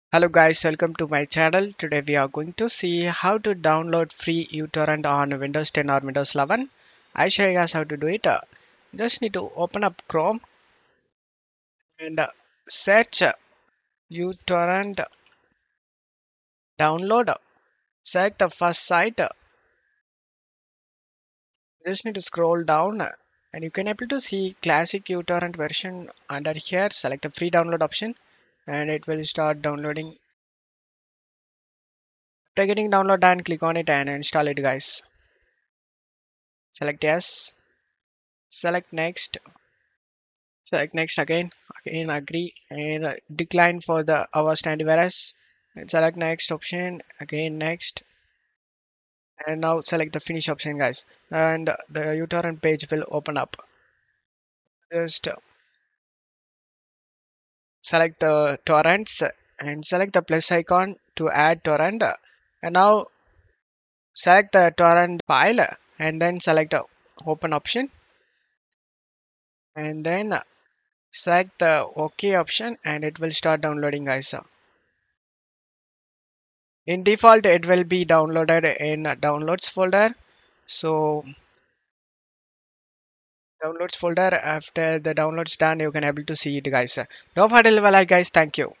How To Download Free uTorrent on Windows 10/11 (2025)
Want to download uTorrent safely and for free in 2025? In this quick and easy guide, I’ll show you how to download and install the official uTorrent client on Windows 10 and Windows 11 without any errors or unwanted software. Works for both 32-bit and 64-bit systems. Watch till the end to avoid fake versions and setup issues!

00:08 How to